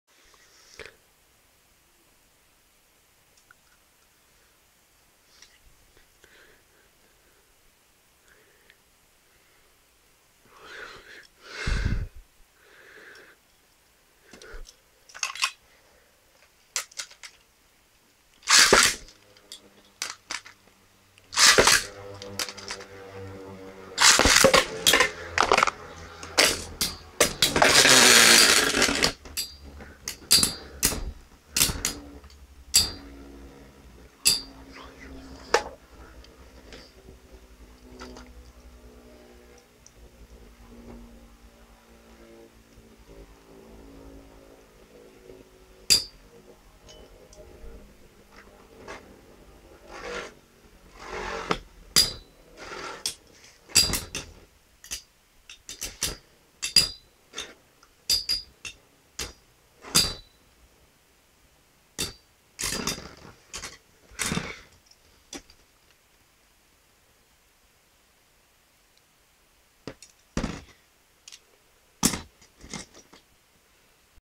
Flame Byxis 230WD vs Ray Unicorno D125CS vs Thermal Lacerta WA130HF
Voici un combat de trois toupies de Metal Master.
Zeo vs Masamune vs Chi-Yun
Bye